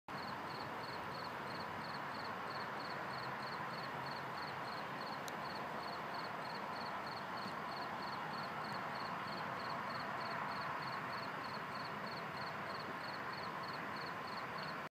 Night at W00
Preflight, file flight plan, get squawk code, listen to crickets...